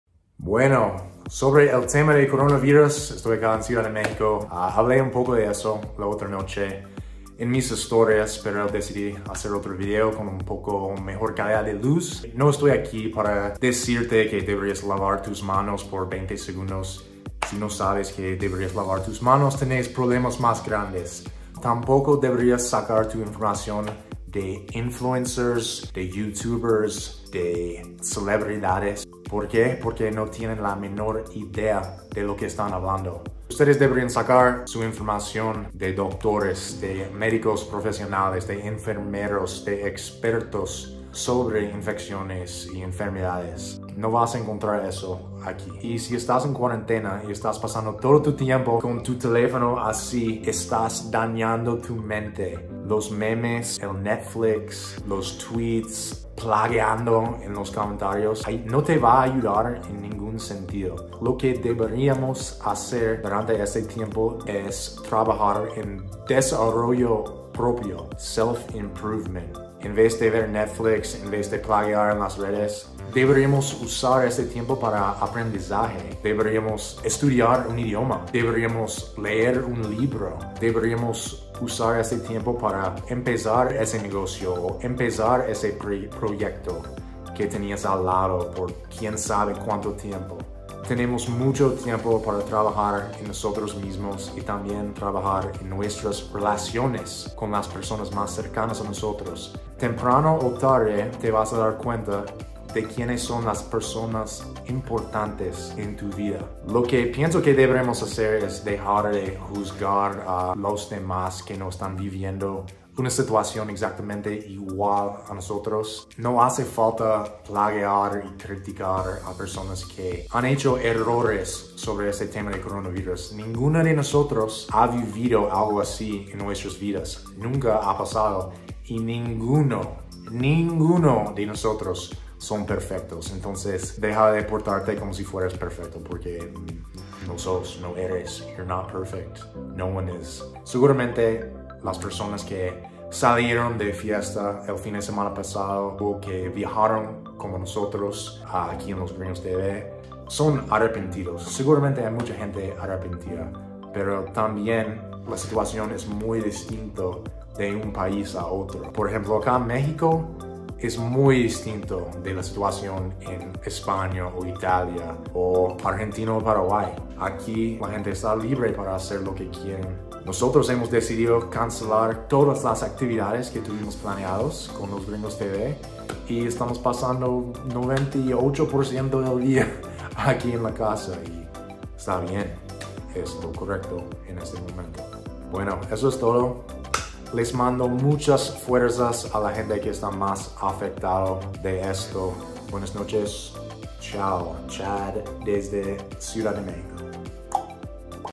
Autodesarollo / Self-Development AHORA! Crisis del V*rus 🦠🙅‍♂️
🦠 ¿Por qué no utilizamos este tiempo que tenemos en cuarentena para trabajar en el autodesarrollo? Es tiempo de cambiar toda esa energía negativa para energía positiva y mejorarnos.
También ha sido difícil para mi seguir con una actitud positiva durante todo esto, pero sigo en la lucha! Ahora mismo estoy estudiando Portugues y Guarani a full! Fuerza a todos!